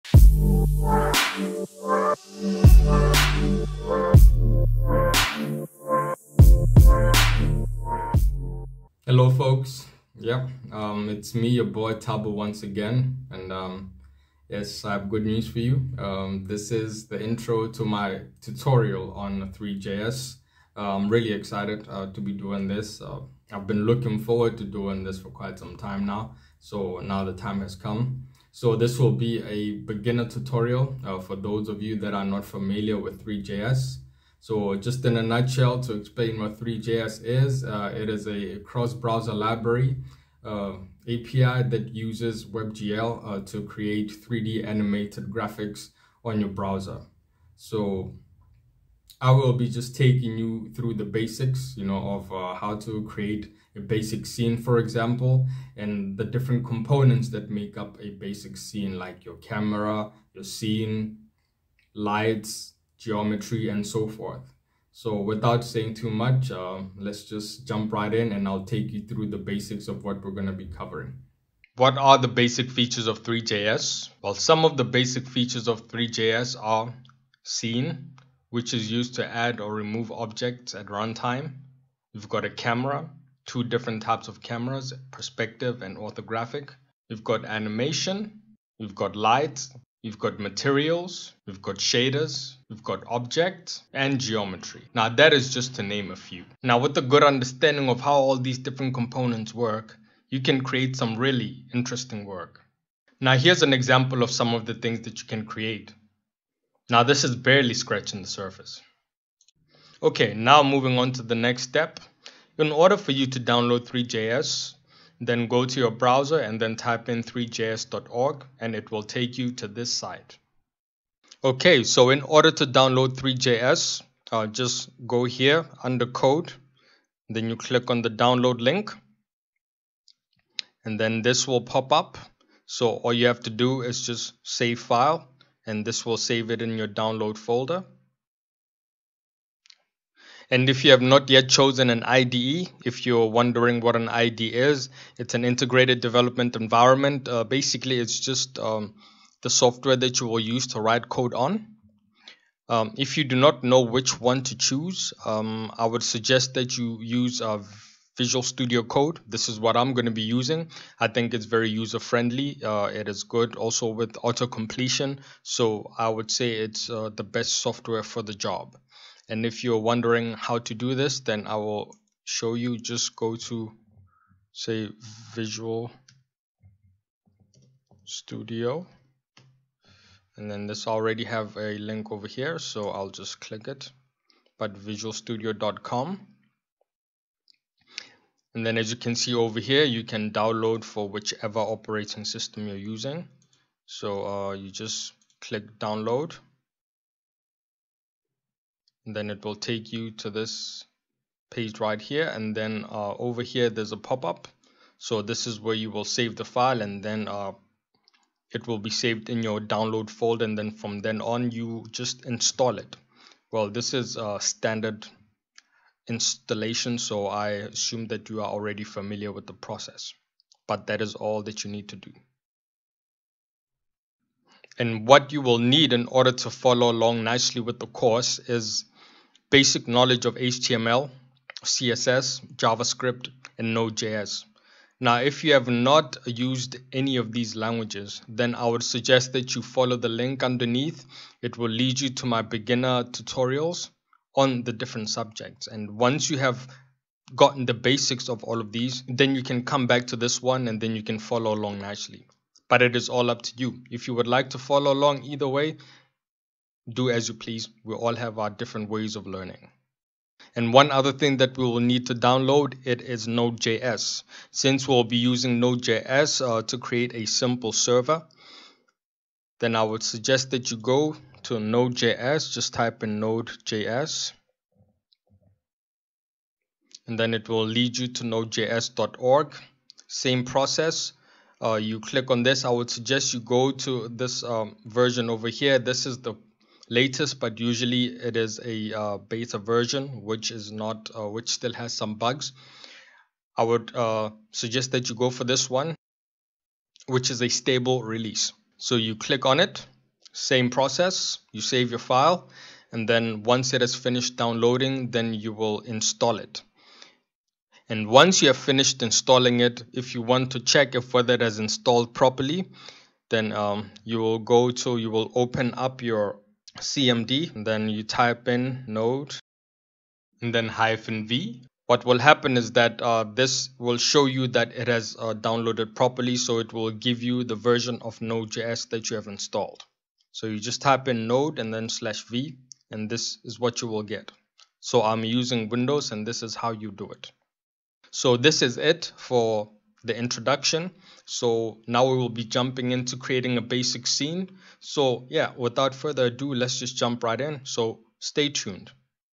Three.js tutorial for absolute beginners: Environment setup
Learn the basic theory on three.js and download the necessary resources to get started.